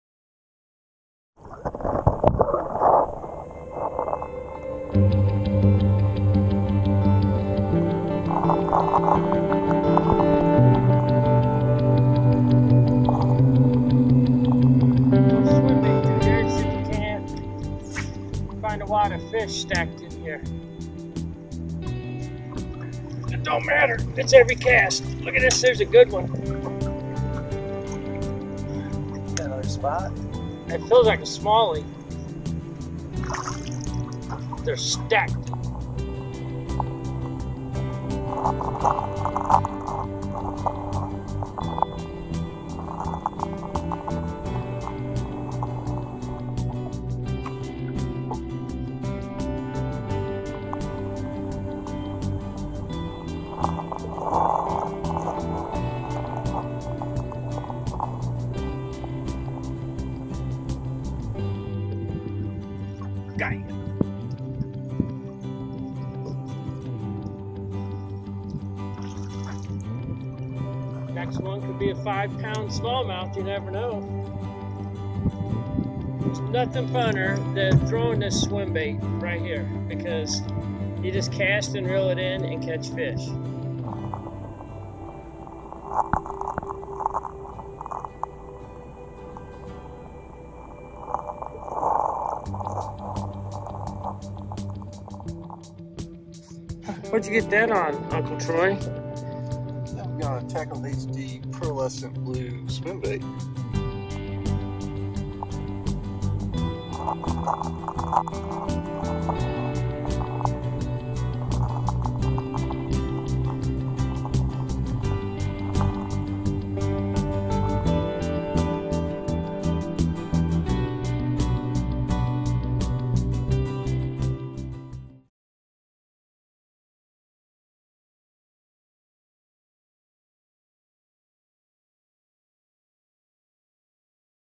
Tackle HD Swimmer Intro
NEW Tackle HD "SWIMMER" swimbait.

The Tackle HD Swimmer is a tournament grade swimbait with unmatched durability and life-like performance! The Swimmer uses the same proprietary plastic formulation used in the Hi-Def Craw that creates neutral buoyancy and tremendous durability. The Tackle HD Swimmer’s design was based on input from our professional staff who outlined the features necessary to produce not only an effective and unique product, but a bait that generates a steady HD swim action proven to trigger strikes when conditions are difficult.

Available in 10 fish catching colors and 3 1/2" and 4" sizes